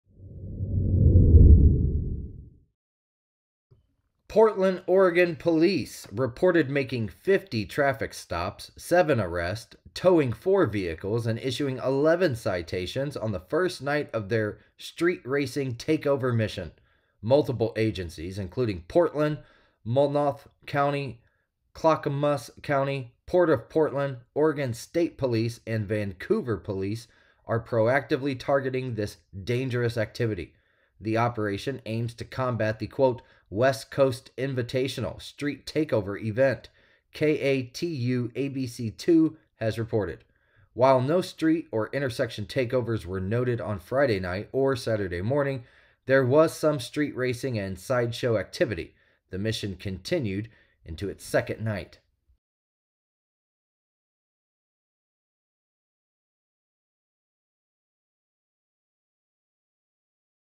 Portland police report 50 traffic stops, 7 arrests in street racing crackdown
Portland Police reported making 50 traffic stops, seven arrests, towing four vehicles, and issuing 11 citations on the first night of their street racing/takeover mission.

Multiple agencies, including Portland, Multnomah County, Clackamas County, Port of Portland, Oregon State Police, and Vancouver Police Department, are proactively targeting this dangerous activity.

The operation aims to combat the “West Coast Invitational” street takeover event, KATU ABC 2 has reported.